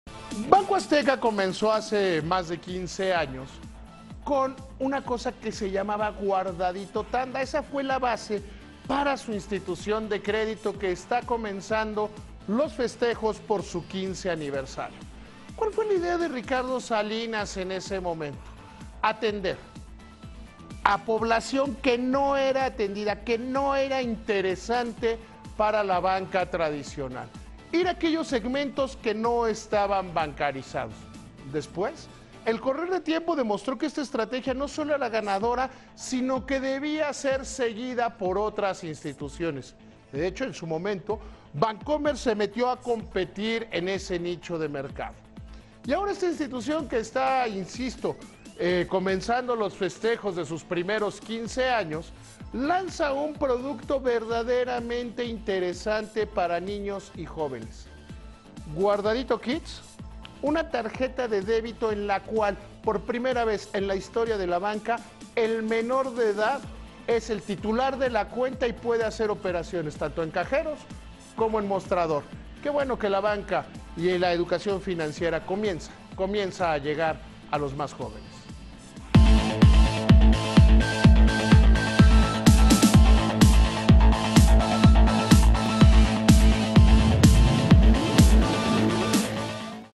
Guardadito celebra 15 aniversario
Hace 15 años Banco Azteca buscó acercarse a las población que no era atendida por instituciones bancarias, sin embargo, Guardadito ha logrado mantenerse en la preferencia de la población.

26 abril de 2017

Te invitamos a ver el siguiente video:

México sí procederá legalmente contra EU por muro: Videgaray